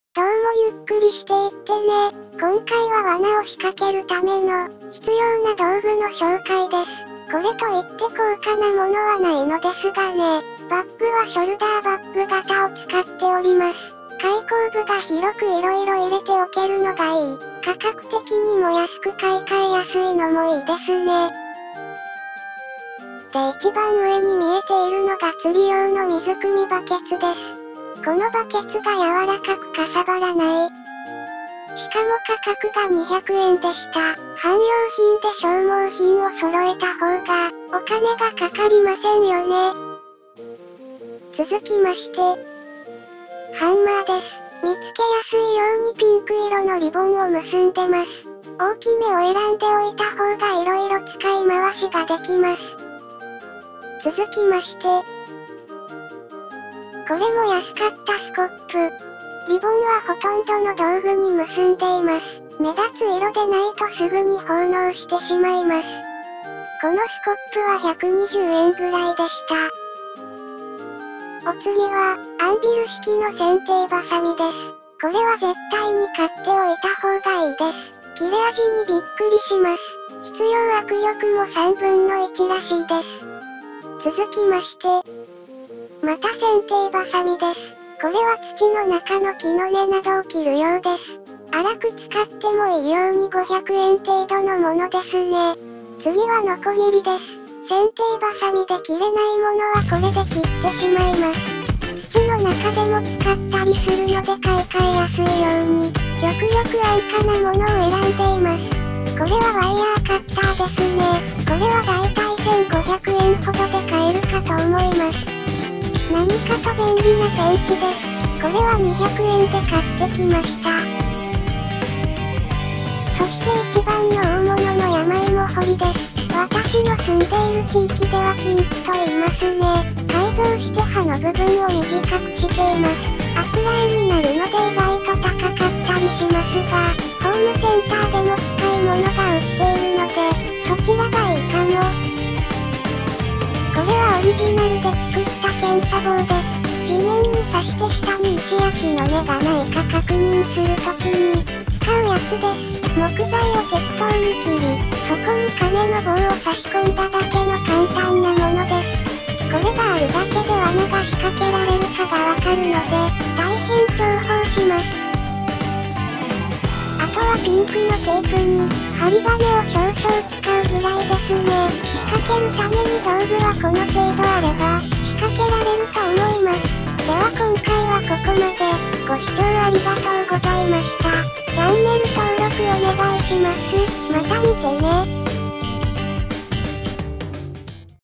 Japanese hunting 狩猟 くくり罠を仕掛けるときに使う １１種類の道具たち ゆっくり狩り動画
くくり罠を仕掛けるときに必要な１１種類の道具です。

ブログもやっていますのでどうぞ！

釣魂クエスト～ボートと魚と能天気な釣り人～